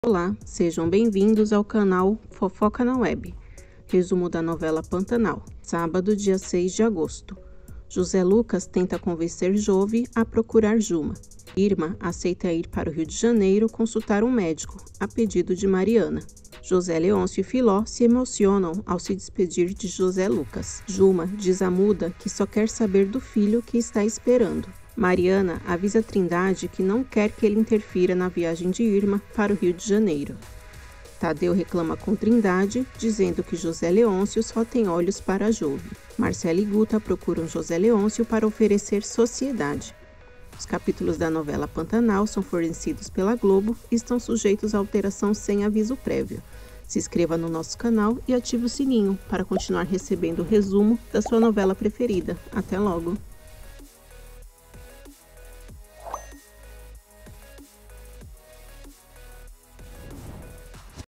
Resumo Da Novela Pantanal de Hoje Sábado 06/08
Resumo da novela Pantanal.

sábado 06 de agosto

José Lucas tenta convencer Jove a procurar Juma.

 Irma aceita ir para o Rio de Janeiro consultar um médico, a pedido de Mariana.

José Leôncio e Filó se emocionam ao se despedir de José Lucas.

 Juma diz a Muda que só quer saber do filho que está esperando.

 Mariana avisa a Trindade que não quer que ele interfira na viagem de Irma para o Rio de Janeiro.

Tadeu reclama com Trindade, dizendo que José Leôncio só tem olhos para Jove.

Marcelo e Guta procuram José Leôncio para oferecer sociedade.

Os capítulos da Novela Pantanal são fornecidos pela Globo e estão sujeitos a alteração sem aviso prévio.

Até logo!